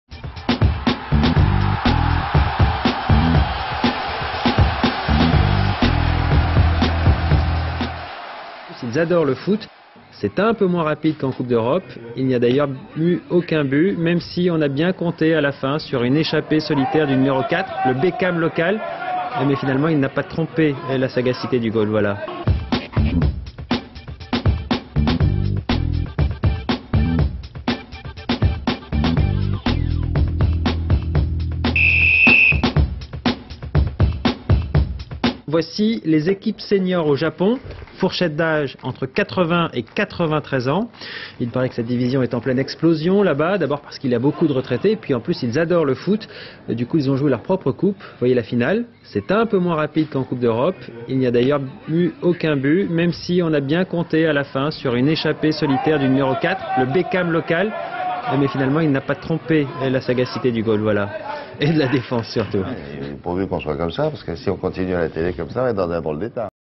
Quiz du foot : numéro 22
29 avril 2010
 Faites appel à votre mémoire ou à votre perspicacité ! Saurez-vous deviner la particularité de cette équipe de footballeurs japonais ? Images d'archive INA